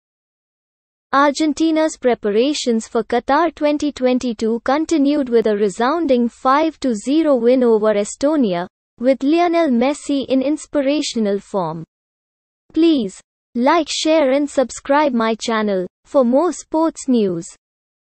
World Cup play-offs Report: Argentina 5-0 Estonia
Argentina's preparations for Qatar 2022 continued with a resounding 5-0 win over Estonia, with Lionel Messi in inspirational form.